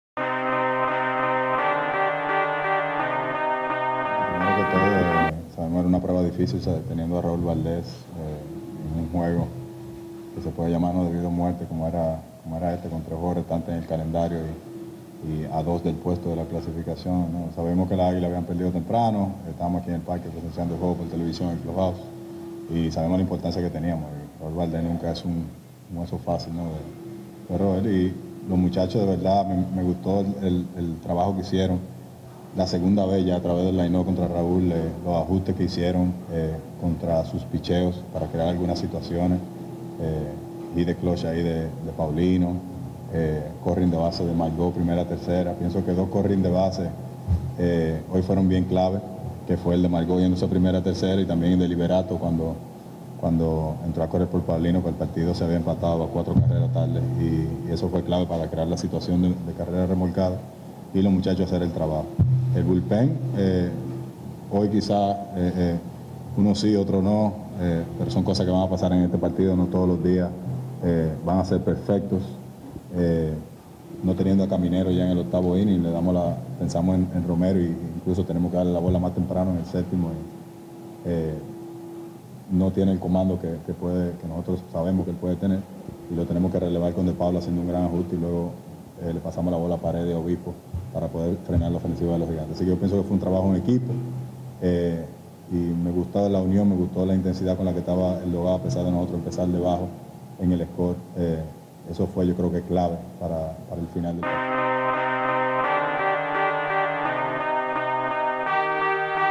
Luis Rojas luego del triunfo de los Leones sobre los Gigantes
El dirigente Luis Rojas en la conferencia de prensa luego de que los Leones vensan a los Gigantes.